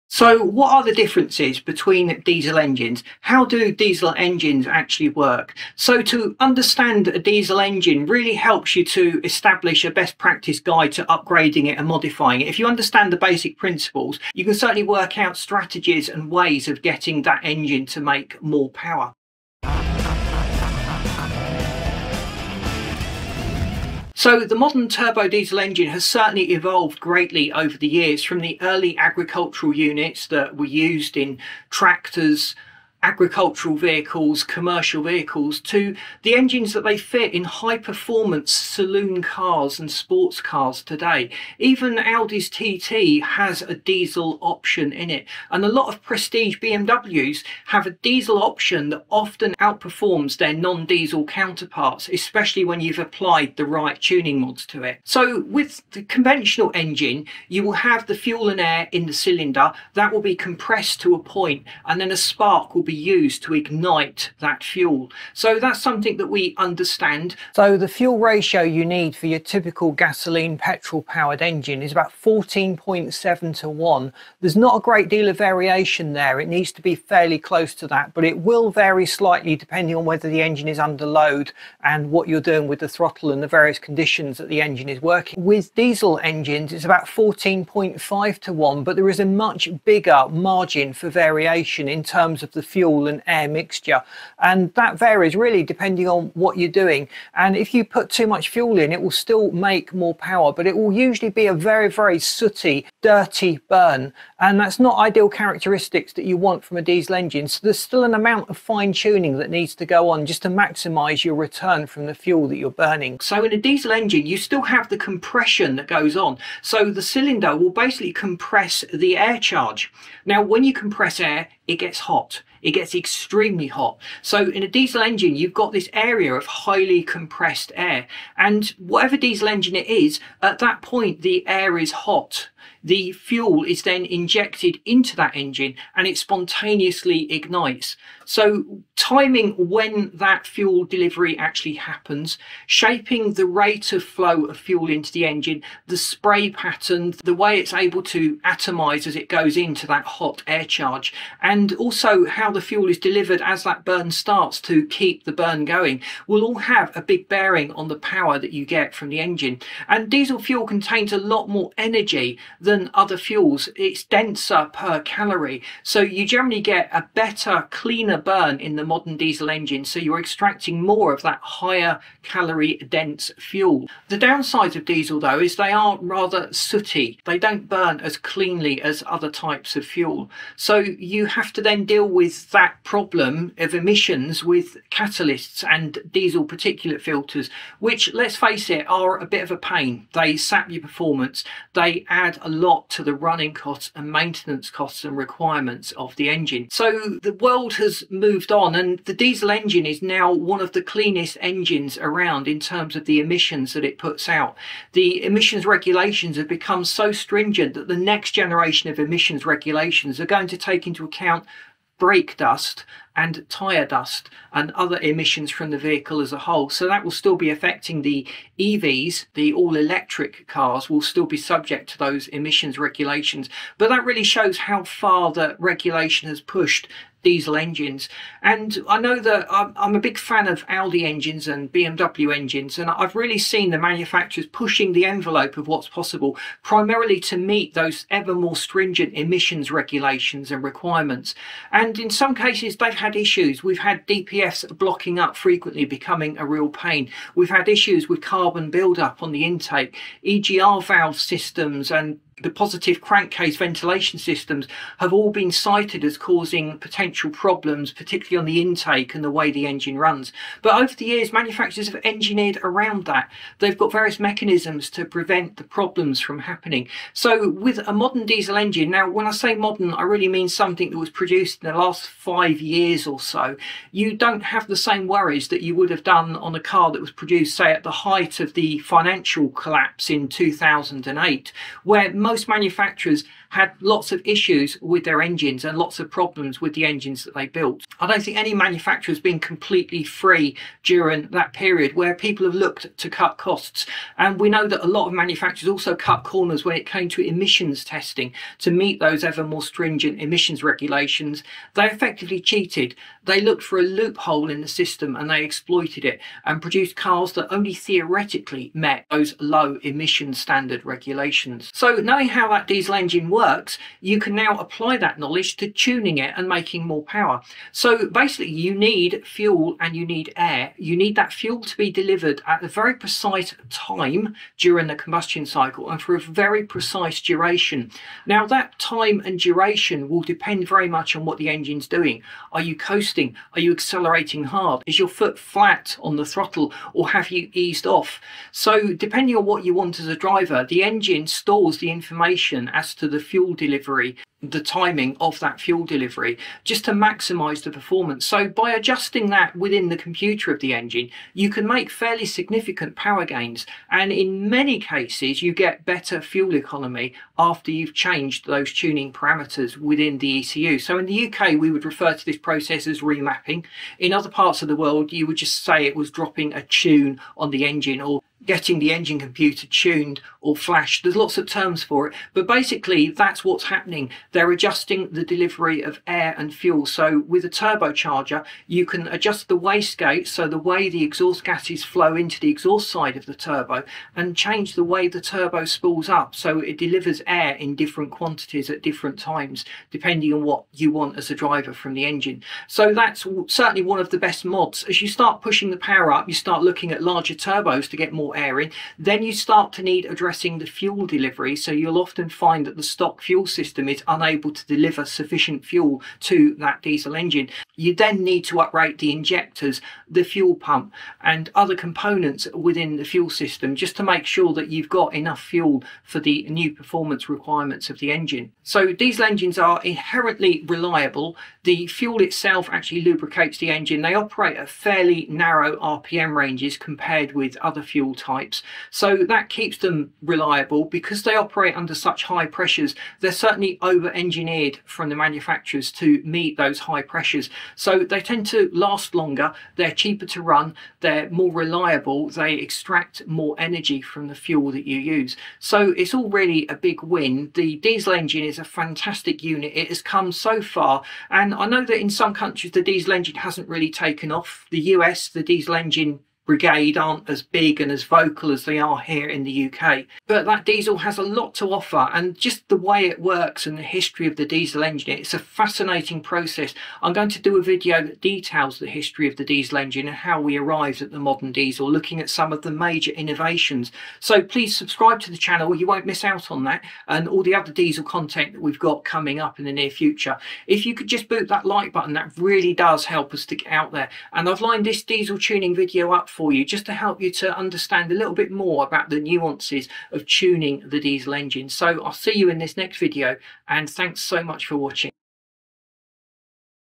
What Are The Differences Between A Diesel Engine And Gas or Petrol Engines? TDI Engines Explained
Enjoy a journey through the remarkable world of diesel engines, uncovering the fascinating differences that contribute to their enhanced power and performance. How do these compare to gasoline (gas engines) aka in some regions as petrol engines?

TDI engines explained - we cover the differences in the diesel engine, how it works and explain the benefits of using diesel engines.

From evolution to innovation, we explore the key factors that make modern diesel engines stand out.

Evolution Of The Modern Diesel Engine
Dive into history and witness the evolution of diesel engines. How have advancements over the years transformed these powerhouses into the modern marvels we know today?

Discover the pivotal changes that have shaped diesel technology.

What Happens In A Conventional Engine?
Explore the inner workings of a conventional engine and delve into the combustion process.

What sets diesel engines apart from their gasoline counterparts, and how do these differences result in more power? Unravel the secrets behind diesel's unique performance.

Fuel & Air Ratios For Engines
Delve into the heart of combustion efficiency. How do diesel engines achieve optimal fuel and air ratios, and what impact does this have on power production?

Gain insights into the delicate balance that drives diesel's power gains.

Factors That The Next Generation Of Emissions Regulations Will Take Into Account
Peek into the future and anticipate the next wave of emissions regulations.

What are the factors driving innovation in diesel technology? Explore the cutting-edge solutions that will shape the next generation of diesel engines.

Modern Diesel Engines Are Very Advanced
Witness the sophistication of modern diesel engines. What advanced technologies are integrated to enhance performance and efficiency?

Delight in the marvels of engineering that push diesel engines to new heights.

Remapping / Engine Tuning
Uncover the potential of remapping and engine tuning. How can these techniques optimize a diesel engine's power output? Explore the art of tuning and its role in harnessing diesel's latent power.

Upgrading The Turbocharger & Fuel System

Journey into the realm of upgrades. How do turbocharger enhancements and fuel system upgrades amplify diesel performance?

Discover how these modifications unlock a world of power potential.

Benefits Of Diesel Engines
Sum up the advantages of diesel engines in a world seeking efficiency and power.

What are the benefits that make diesel a preferred choice for certain applications? Gain a comprehensive understanding of the strengths that set diesel engines apart.

Join us on this captivating expedition as we peel back the layers of diesel differences, revealing the intricate details that contribute to their heightened power and efficiency.

By exploring the evolution, combustion nuances, technological advancements, and tuning possibilities, you'll gain a newfound appreciation for the diesel engine's prowess.

0.00 Start
0.19 Intro
0.26 Evolution Of The Modern Diesel Engine
0.56 What Happens In A Conventional Engine?
1.07 Fuel & Air Ratios For Engines
3.28 Factors That The Next Generation Of Emissions Regulations Will Take Into Account
4.39 Modern Diesel Engines Are Very Advanced
5.50 Remapping / Engine Tuning
6.35 Upgrading The Turbocharger & Fuel System
7.21 Benefits Of Diesel Engines
7.54 Conclusion